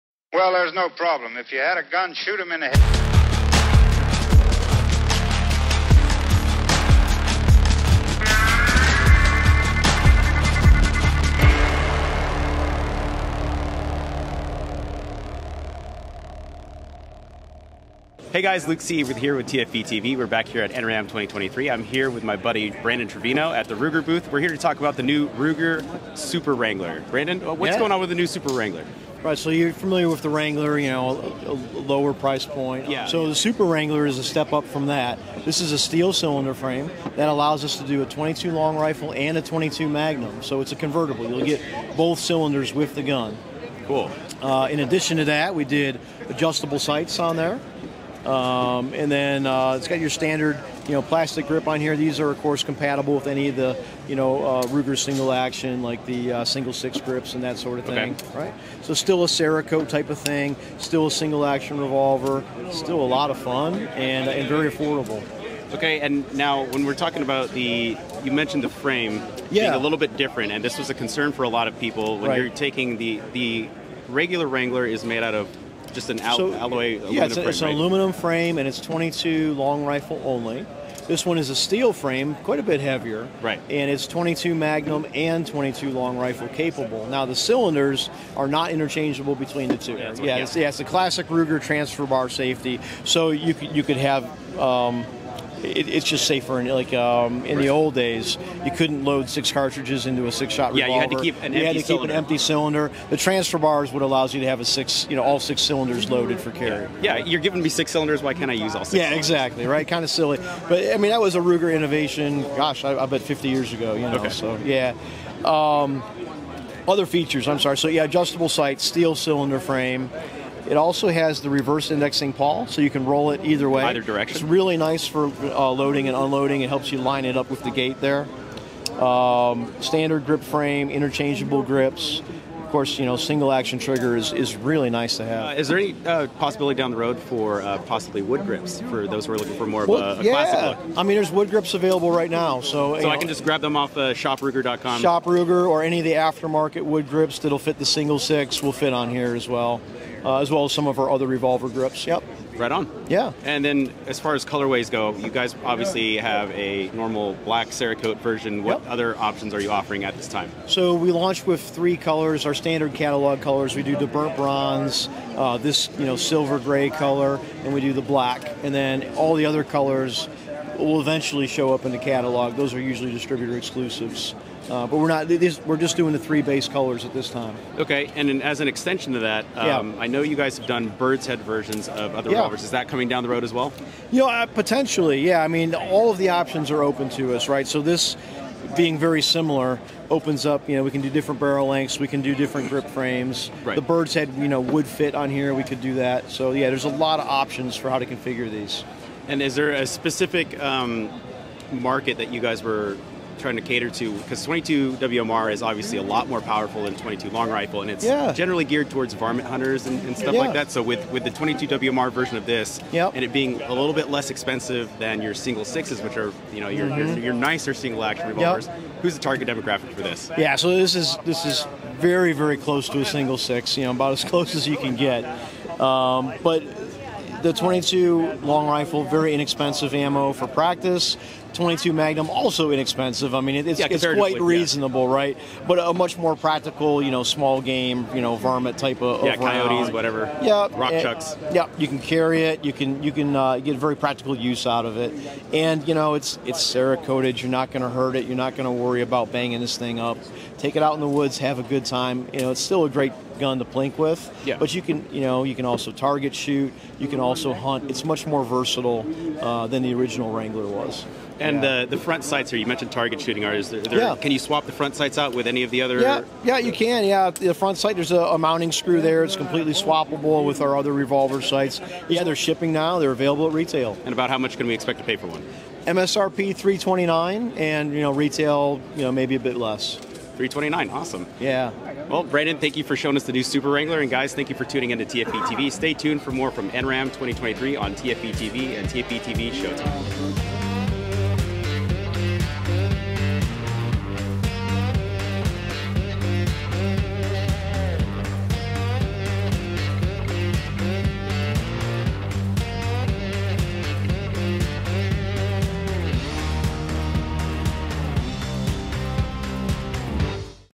The New 22 Magnum Ruger Super Wrangler
Luke C. is stopping by the Ruger booth at NRAAM 2023 to check out the new Ruger Super Wrangler that was released just shortly before the start of the NRA Annual Meetings. The New Ruger Super Wrangler combines some of the best features of the more expensive Single Six line of revolvers and the ruggedness and pricing of the Wrangler series to give customers a versatile plinking, hunting, and target-shooting single-action revolver.